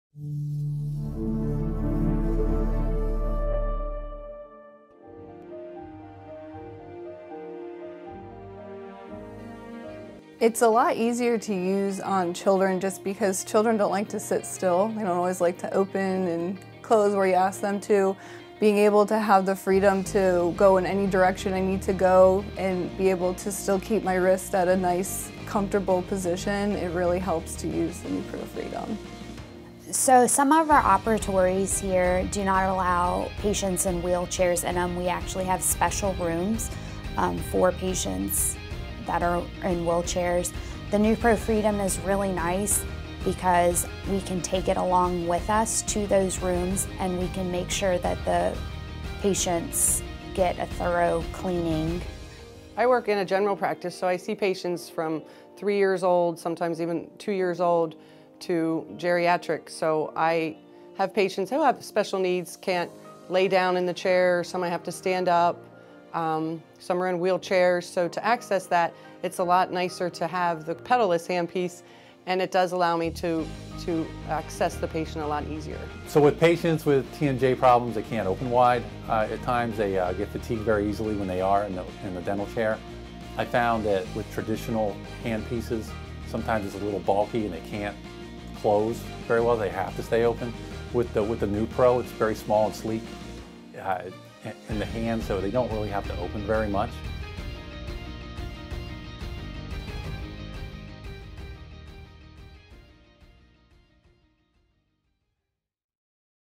Nupro Freedom Pediatrics and Special Cases | Dentsply Sirona
The Nupro Freedom is a valuable tool for pediatric offices and those that deal with special cases. Quietness, control and portability are excellent features when polishing pediatric patients.